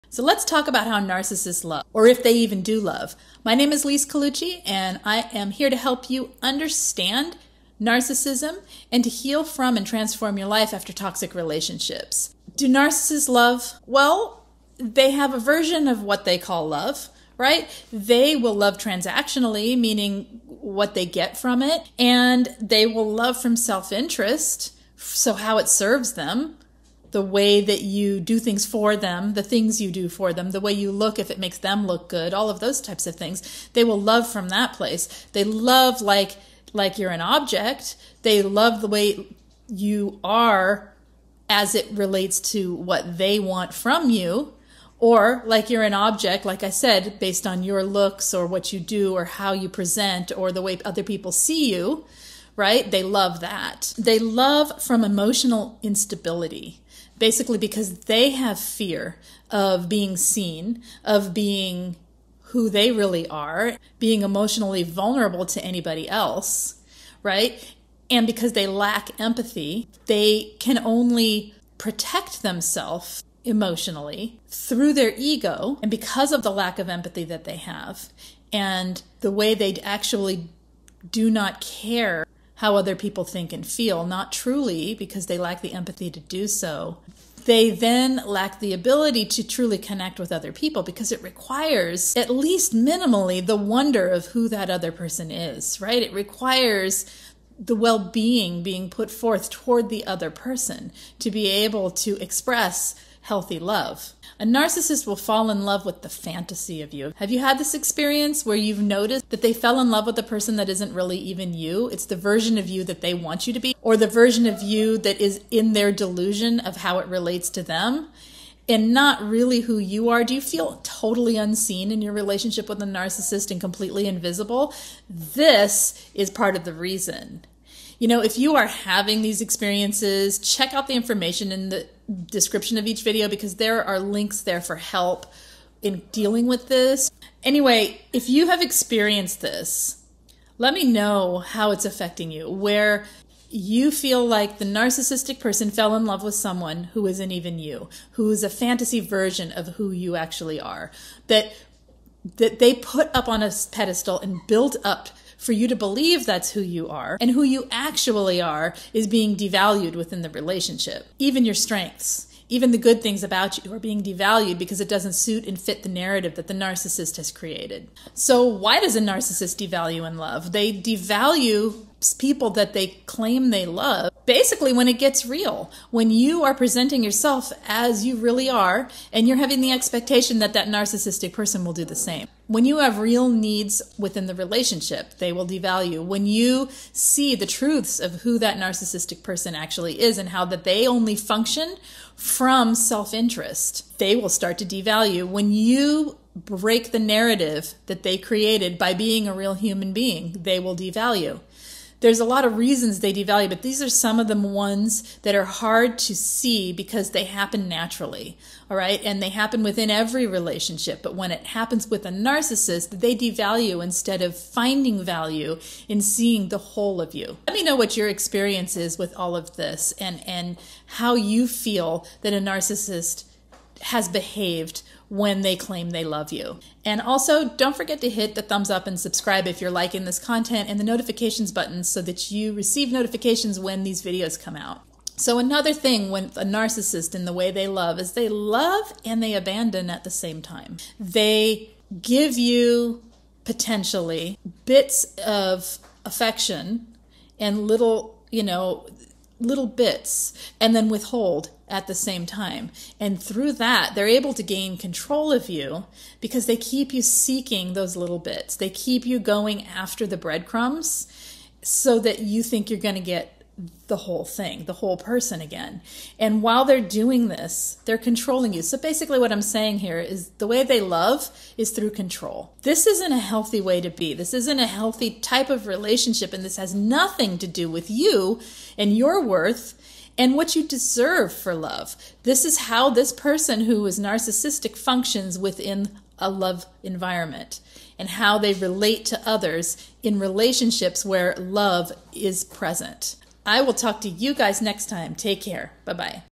Love From A Narcissist Is Devaluing And Toxic
Love Of A Narcissist Is Devaluing And Toxic
Narcissists' love is toxic. At first, they can be super affectionate, making you feel really special. But it's mainly about getting attention and control. As time goes on or if they feel threatened, their love can turn cold. They find it hard to give real, selfless love because they're so self-centered. So, their relationships often go through a cycle of making you feel amazing one moment and then devaluing you the next. It leaves you feeling confused and always looking for their approval. Through it all is a self centered narcissist who lacks the ability to have deep connections and has no empathy for the person who they claim to love.